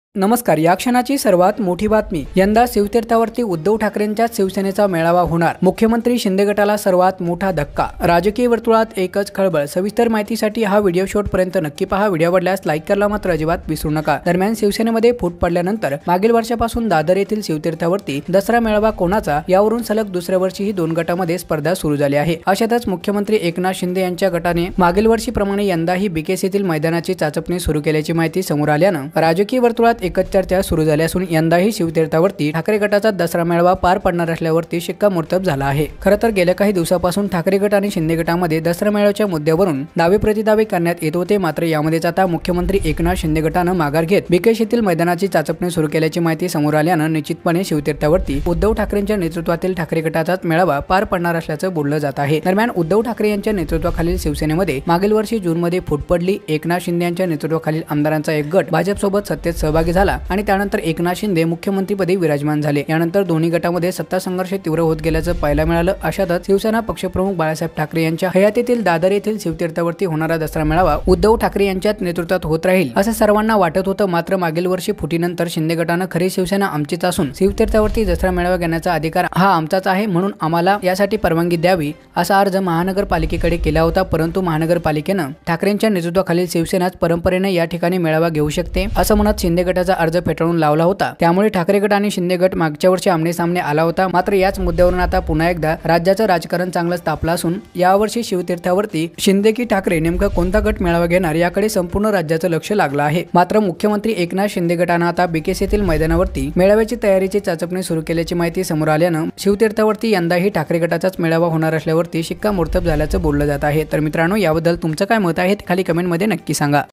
शिंदे गटाची माघार ठाकरेंनी मारली बाजी शिवतीर्थावर या वर्षी सुध्दा ठाकरेंचाच मेळावा ! latest news
शिंदे गटाची माघार ठाकरेंनी मारली बाजी शिवतीर्थावर या वर्षी सुध्दा ठाकरेंचाच मेळावा ! latest news today MH